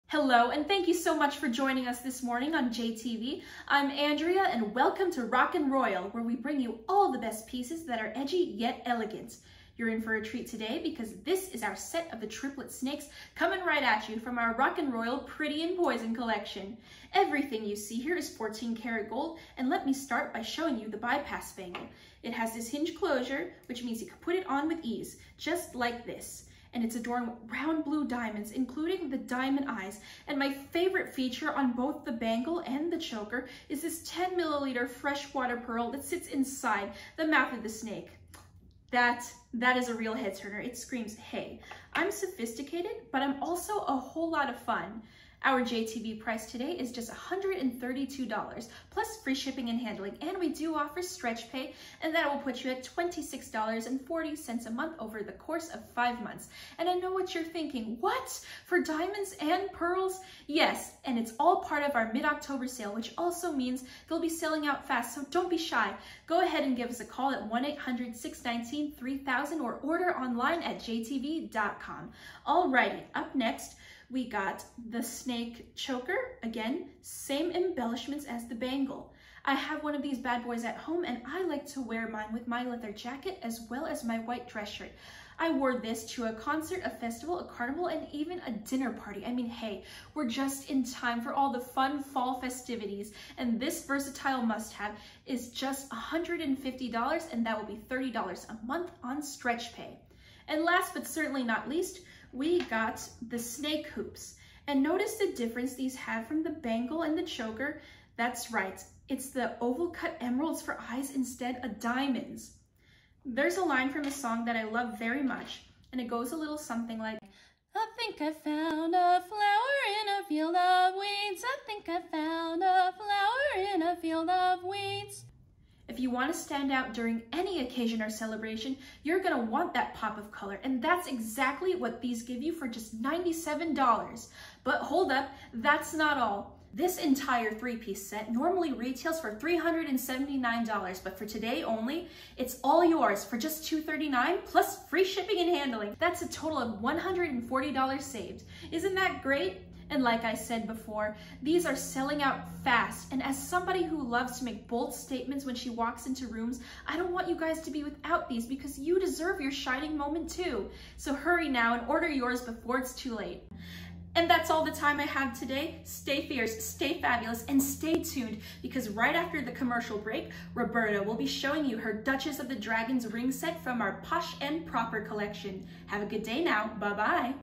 JTV Audition Andrea Domo Ibea
One of my wildest career dreams is to be a JTV Host, because I want to talk about all the cool pieces of Jewlery that can bring an entire look to life. I enjoy styling myself and friends and would be delighted to help others have their shining moment too!

I hope you all enjoy this video I put together. It was a lot of fun to make!

Yours Truly,

Andrea Domo-Ibea